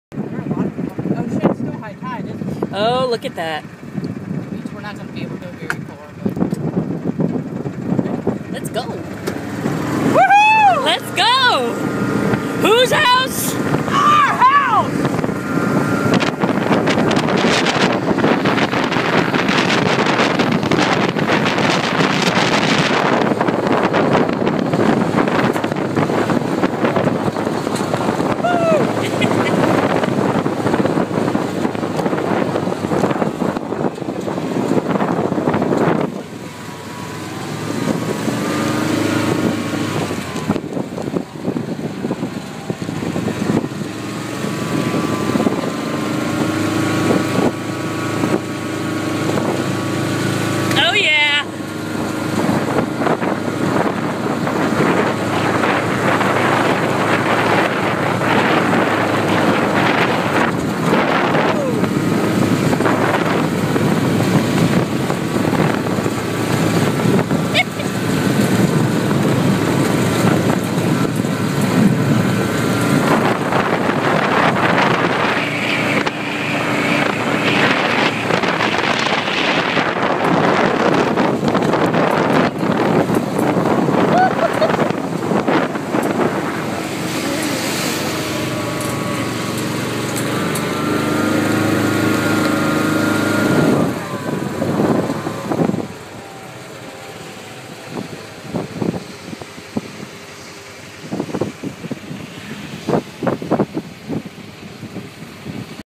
Tangier Island Beach
A ride on Tangier Island beach in a Kawasaki Mule 610XC.